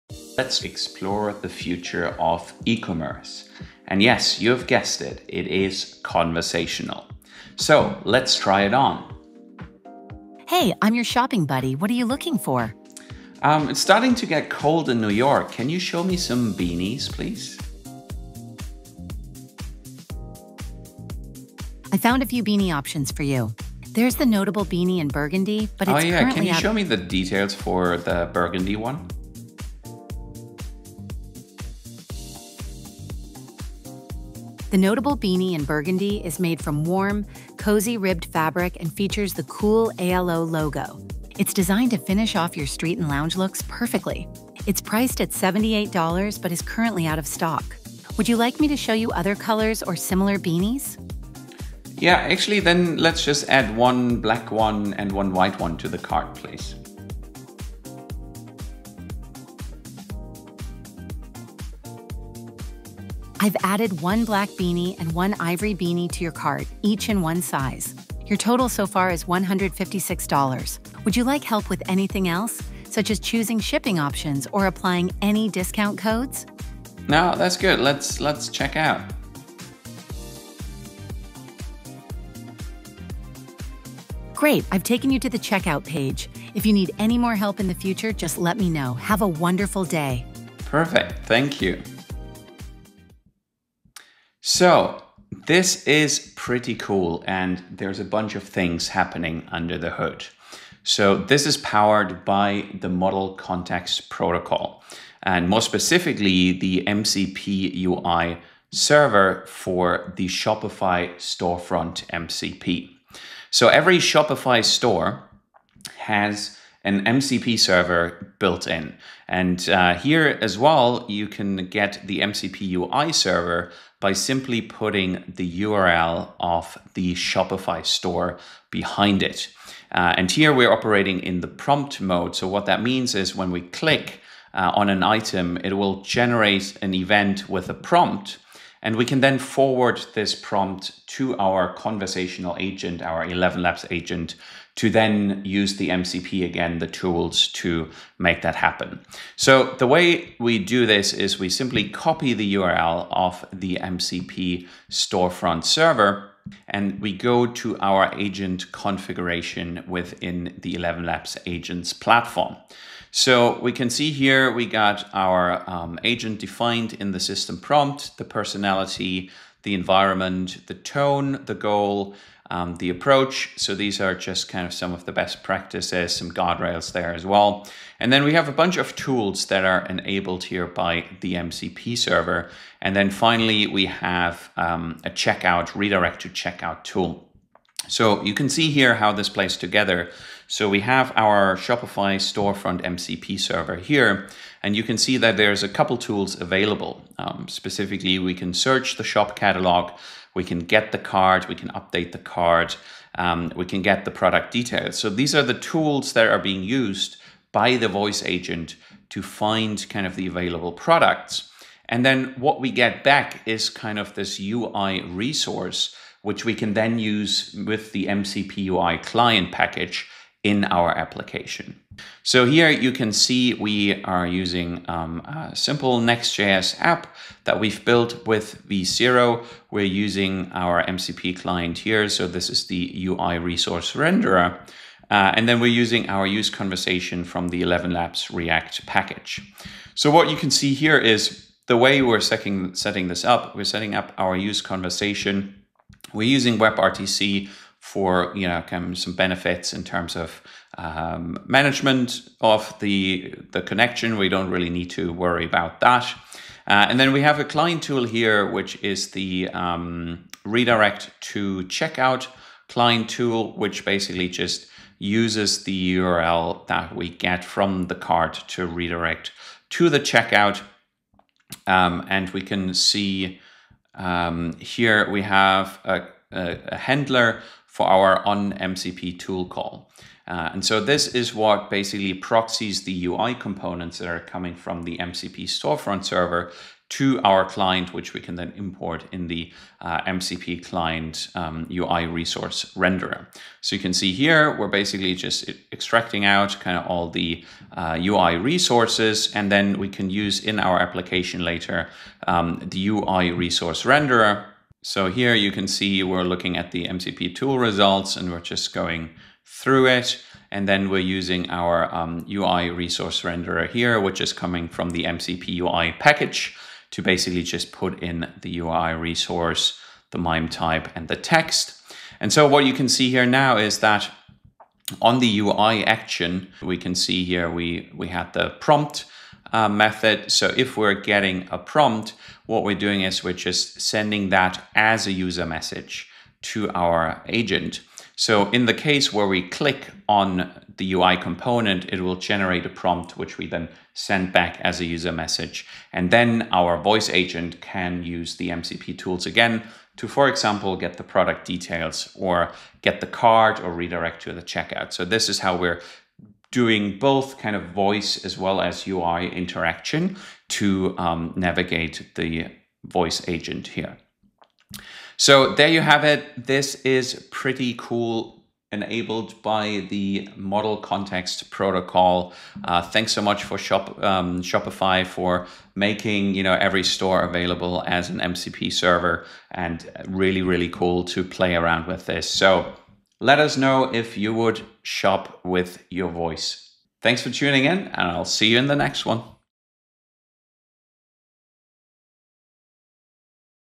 Voice-Powered Shopping with ElevenLabs and Shopify
Explore the future of online shopping powered by conversational AI from ElevenLabs. In this demo, you’ll see how a natural voice assistant transforms the e-commerce experience — helping a shopper browse, choose, and check out completely through conversation.

From asking for cozy beanies to completing checkout, this demo shows how AI voices, speech recognition, and contextual understanding can create frictionless, human-like shopping experiences.

Whether you’re an e-commerce brand, developer, or AI enthusiast, this tutorial gives you a glimpse into the next era of voice-enabled retail.

## Chapters
00:00 – Introduction: The future of e-commerce
00:06 – What makes shopping conversational
00:12 – The AI shopping assistant begins
00:15 – User request: Finding a beanie for cold weather
00:25 – Exploring available beanie options
00:28 – Product details: The Notable Beanie
00:38 – Material, design, and price overview
00:48 – Availability and color variants
00:52 – Adding black and ivory beanies to the cart
01:03 – Cart summary and pricing
01:13 – Checkout and discount options
01:17 – Completing the purchase
01:27 – Final confirmation and closing interaction
01:34 – Technical overview: how the system operates
01:41 – Closing remarks: conversational AI in commerce